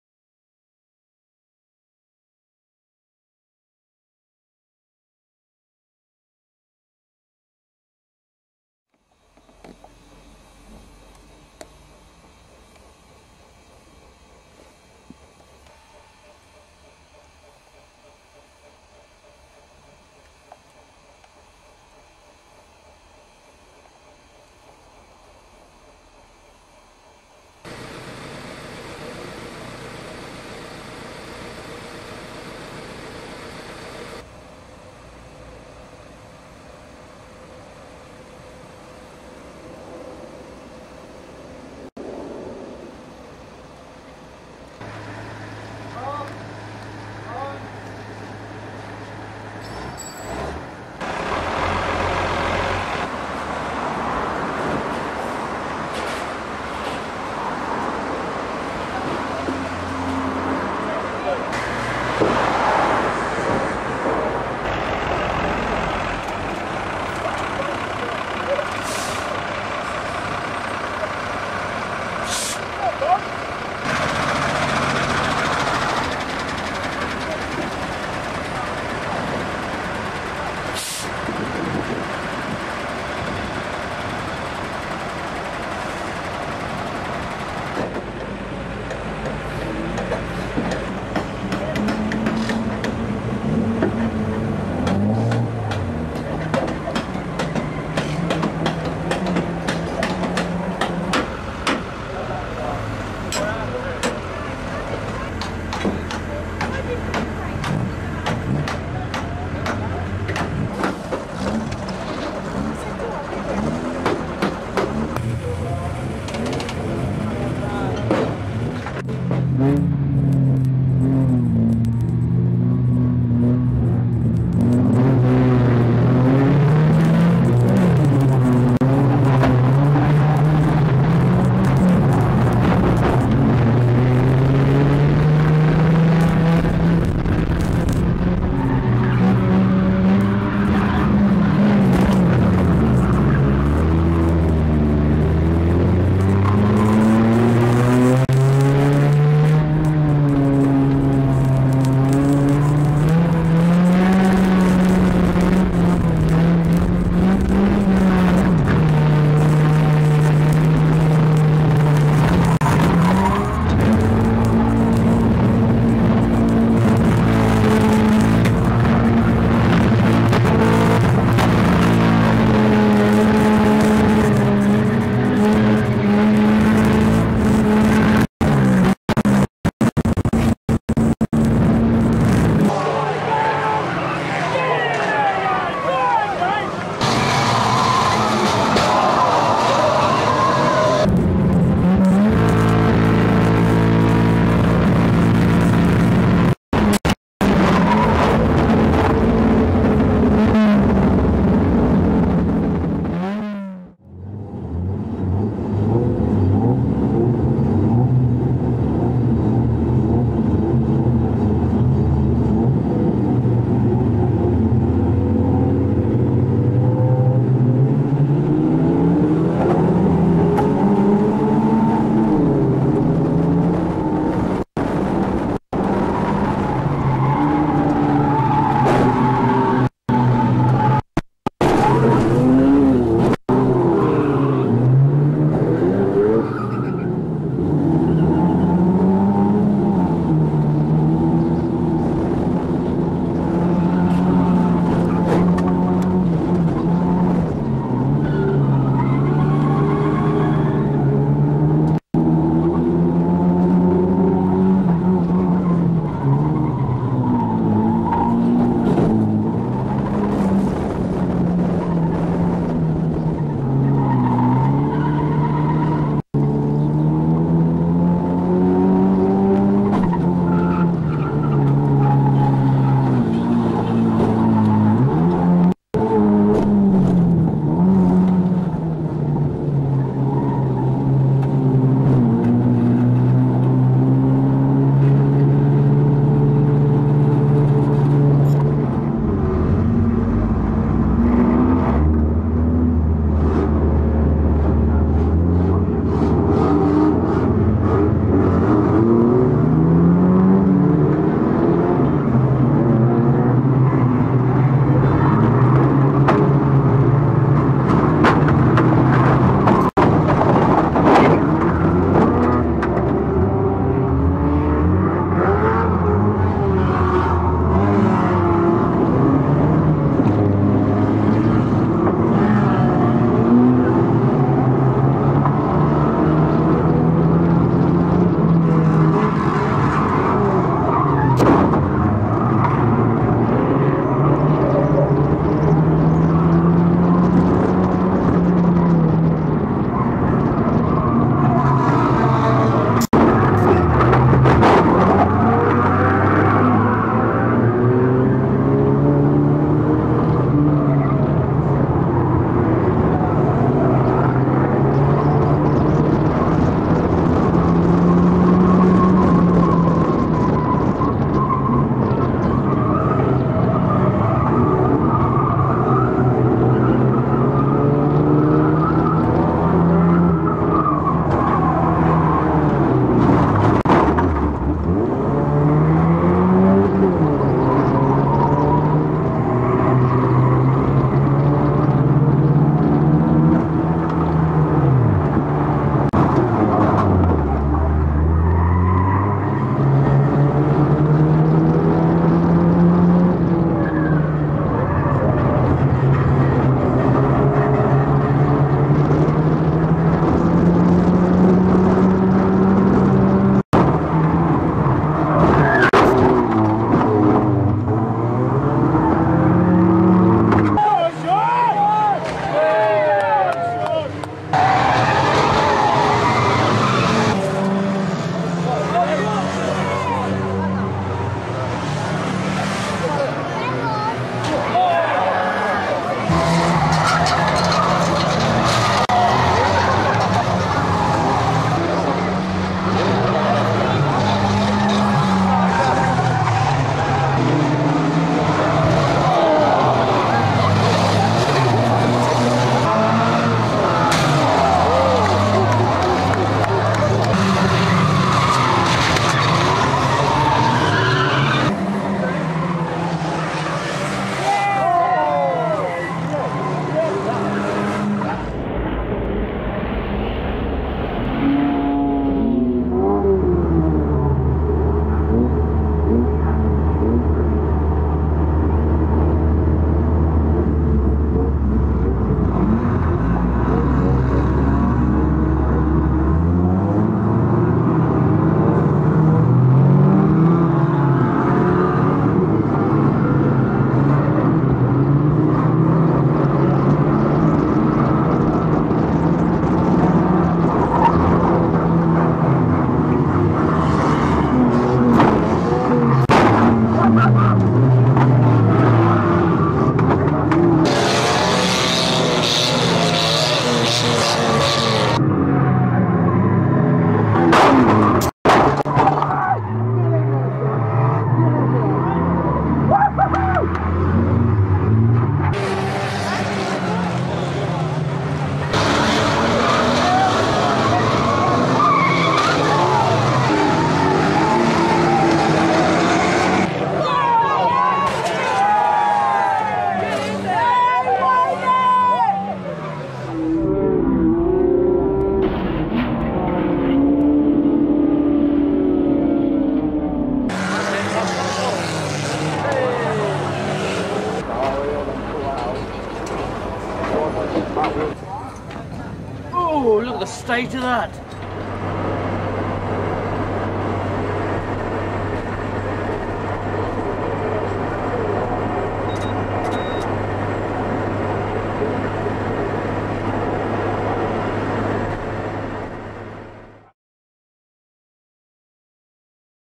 Team Aftermath at St Day Easter 2006 Impact Videos
An example of the action from our Team Aftermath Profile DVDs featuring the teams crashing exploits around the country. This is a road trip from Easter 2006 where the team travel to Cornwall for the St Day Boneshayker after having raced at Mildenhall on Good Friday and back to Kings Lynn for Easter Monday.